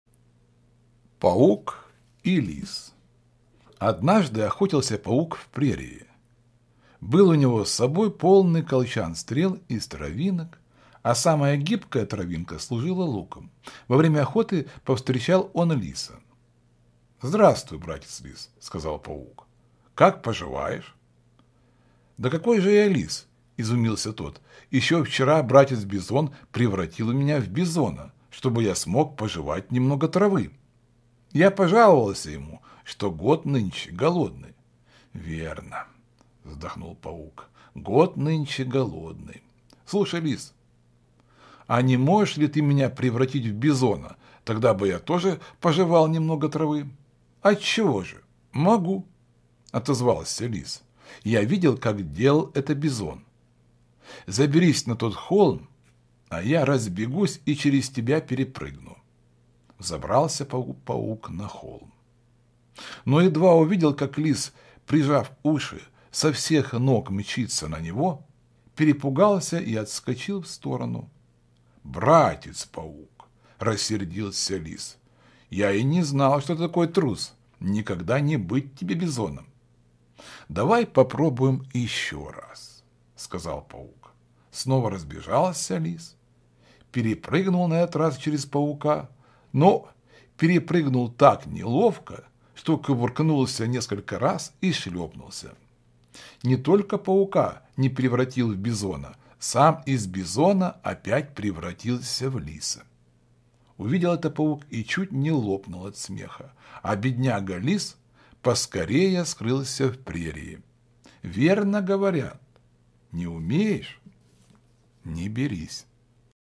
Паук и Лис, Притча, читает Павел Беседин
Однажды охотился Паук в прерии. Был у него с собой полный колчан стрел из травинок, а самая гибкая травинка служила луком. Во время охоты повстречал он Лиса.

— Здравствуй, братец Лис, — сказал Паук. — Как поживаешь?

— Да какой же я Лис, — изумился тот. — Еще вчера братец Бизон превратил меня в Бизона, чтобы я смог пожевать немного травы. Я пожаловался ему, что год нынче голодный.

— Верно, — вздохнул Паук, — год нынче голодный.

Слушай, Лис, а не можешь ли ты меня превратить в Бизона, тогда бы я тоже пожевал немного травы.

— Отчего же, могу, — отозвался Лис. — Я видел, как делал это Бизон. Заберись на тот холм, а я разбегусь и через тебя перепрыгну.

Взобрался Паук на холм. Но едва увидел, как Лис, прижав уши, со всех ног мчится на него, перепугался и отскочил в сторону.

— Братец Паук, — рассердился Лис, — я и не знал, что ты такой трус. Никогда не быть тебе Бизоном.

— Давай попробуем еще раз, — сказал Паук.

Снова разбежался Лис, перепрыгнул на этот раз через Паука, но перепрыгнул так неловко, что кувырнулся несколько раз и шлепнулся.

Не только Паука не превратил в Бизона — сам из Бизона опять превратился в Лиса.

Увидел это Паук и чуть не лопнул от смеха. А бедняга Лис поскорее скрылся в прерии.

Верно говорят: не умеешь — не берись.